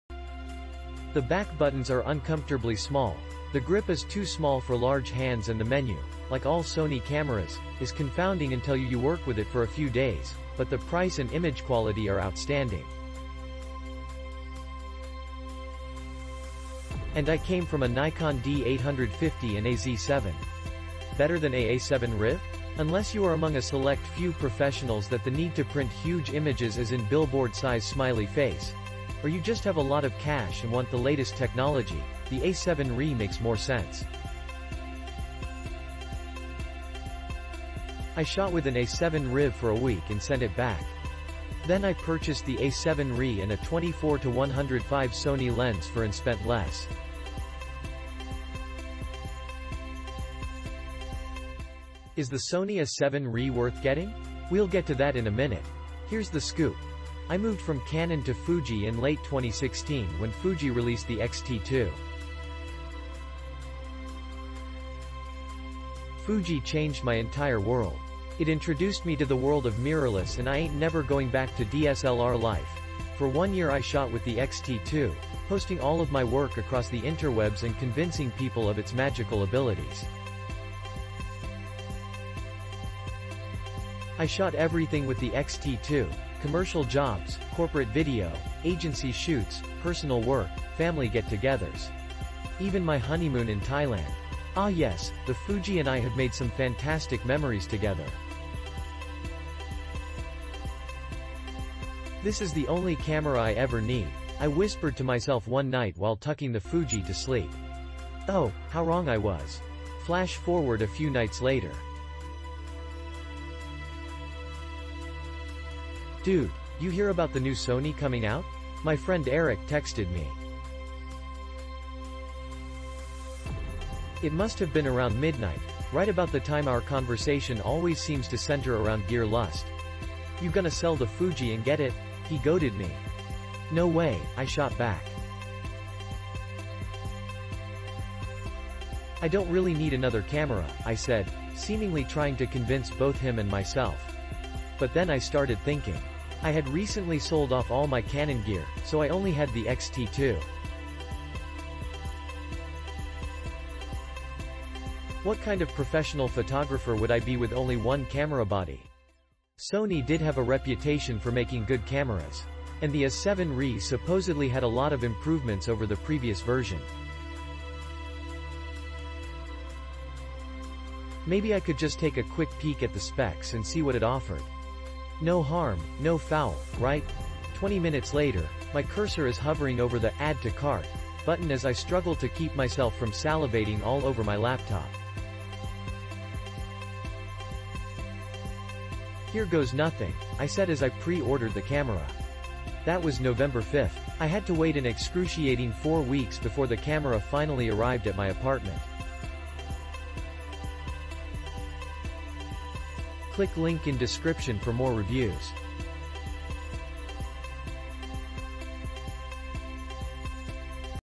More In The Description Sony a7R III Mirrorless Camera: 42.4MP Full Frame High Resolution Inter...
Sony a7R III Mirrorless Camera: 42.4MP Full Frame High Resolution Interchangeable Lens Digital Camera with Front End LSI I...

The back buttons are uncomfortably small, the grip is too small for large hands and the menu (like all Sony cameras) is confounding until you you work with it for a few days, but the price and image quality are outstanding.
And I came from a Nikon D850 and a Z7! Better than a A7riv? Unless you are among a select few professionals that the need to print huge images as in billboard size :))),or you just have a lot of cash and want the latest technology, the A7rIII makes more sense.
I shot with an A7riv for a week...